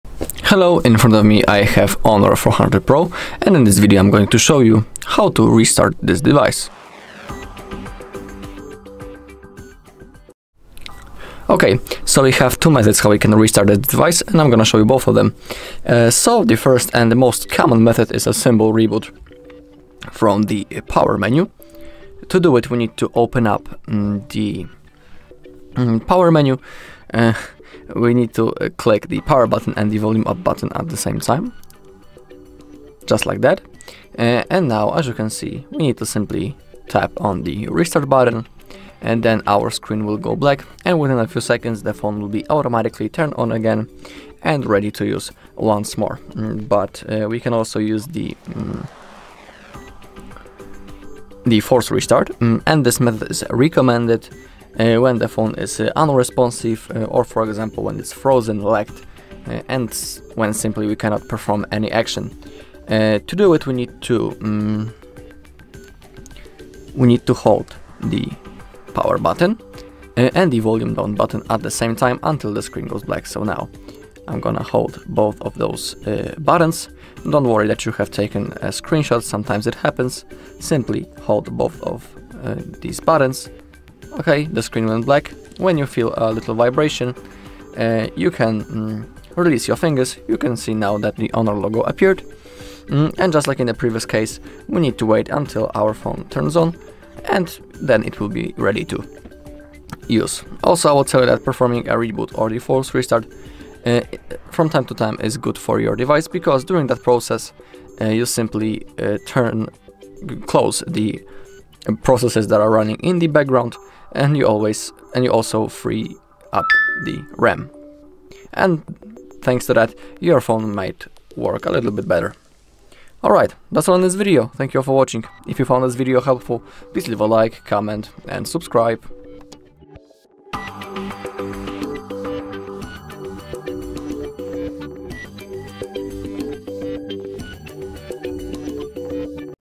HONOR 400 Pro – How to Restart
If your HONOR 400 Pro is running slow, frozen, or you just want to refresh your device, restarting it can help solve many common issues. In this video, you will learn two easy ways to restart your HONOR 400 Pro smartphone. We show you the standard restart using the power menu, as well as the force restart method for situations when your phone is unresponsive or frozen. Both methods are simple and safe, and restarting your device can help close background apps and improve performance. Follow along to see step-by-step instructions for each method on the HONOR 400 Pro.

How to restart HONOR 400 Pro using the power menu?
How to force restart HONOR 400 Pro if it is frozen?
Does restarting HONOR 400 Pro improve performance?

0:00 Introduction
0:08 Standard restart from power menu
0:28 Steps to use the power menu
0:46 Force restart when device is unresponsive
1:13 How to perform a force restart
1:40 Why restarting can help your phone
2:03 Outro